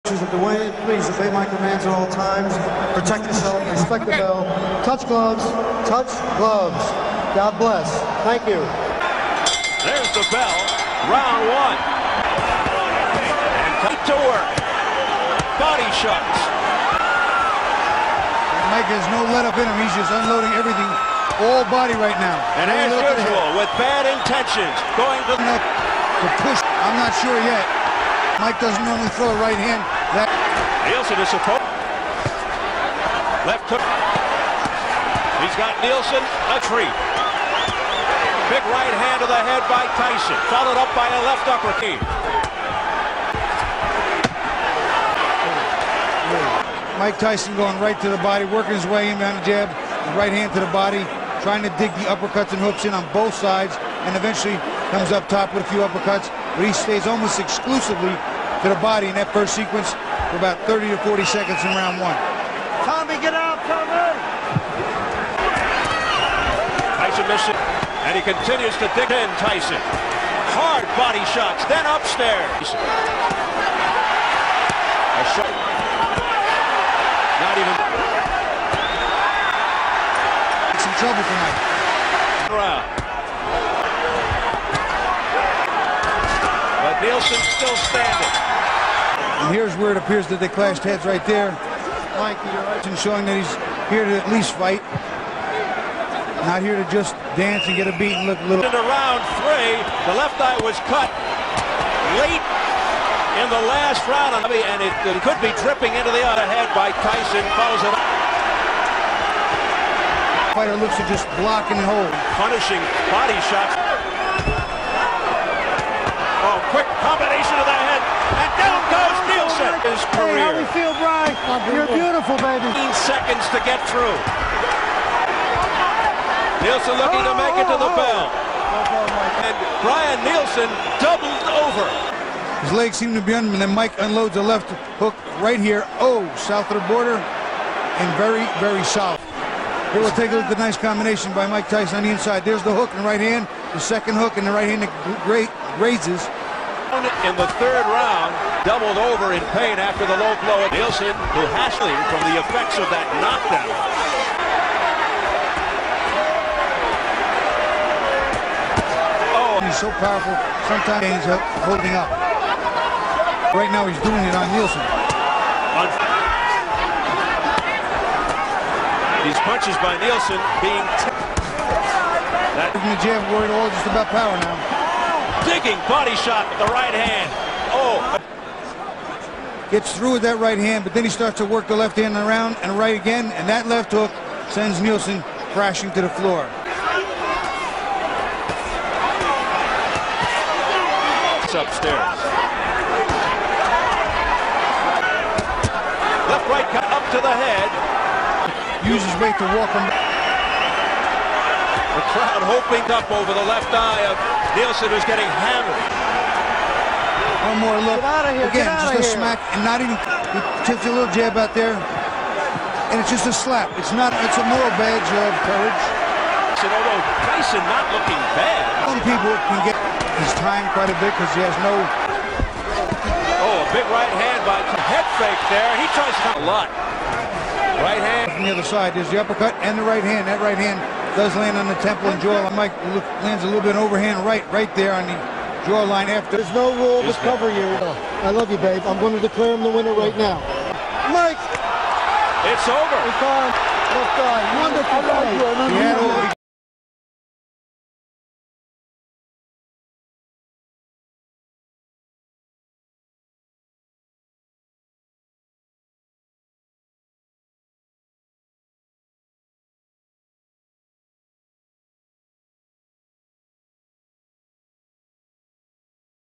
Mike Tyson (USA) vs Brian Nielsen (Denmark) | Boxing Highlights | HD
Watch the thrilling highlights of the bout between Mike Tyson and Brian Nielsen in stunning HD! Witness Tyson's unmatched power and aggression as he faces the resilient Danish fighter, Nielsen, in a match filled with intense action and dramatic moments. Relive the key exchanges and unforgettable highlights that showcase Tyson's legendary prowess in the ring.